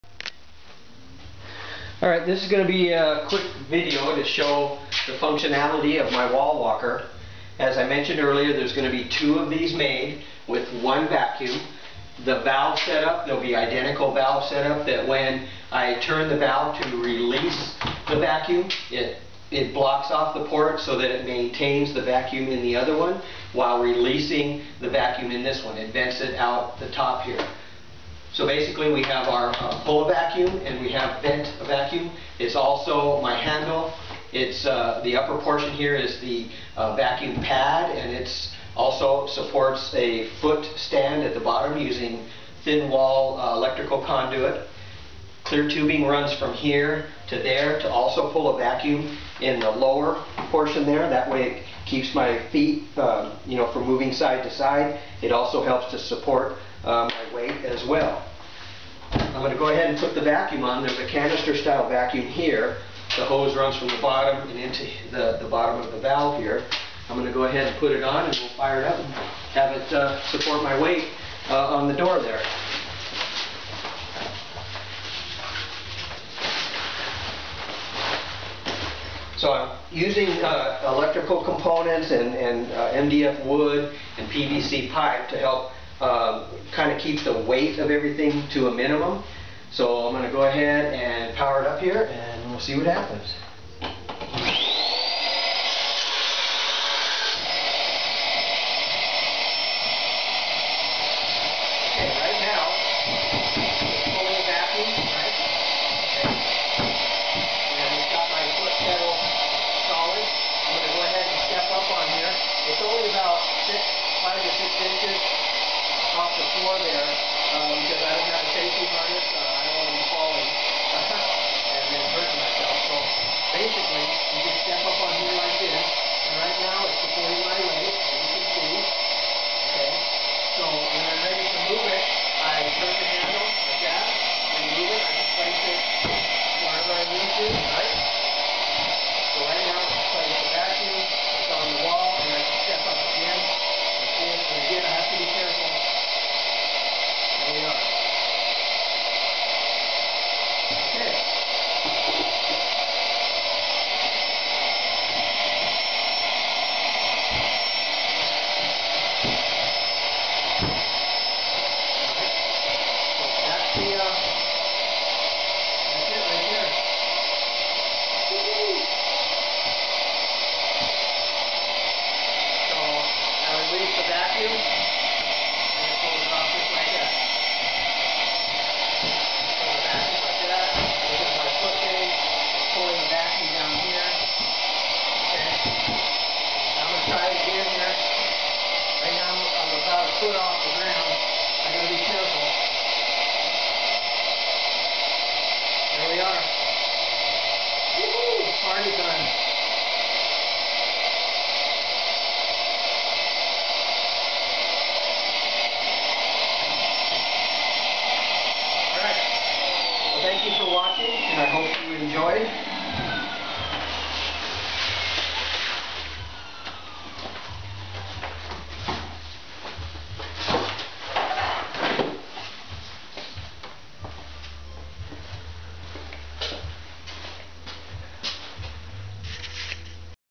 vacuum powered wall climber 1 of 2
Custom made vac. valve, vent and handle. Vacuum wall climber 1 of 2.
My own design and functionality. The main difference is the design of my valve system and the use of one vacuum instead of two vacuums as others have done.
This is a one of a kind setup with a vacuum pad for the foot as well. This design with the foot pad also helps to support the entire weight of myself and the single vacuum.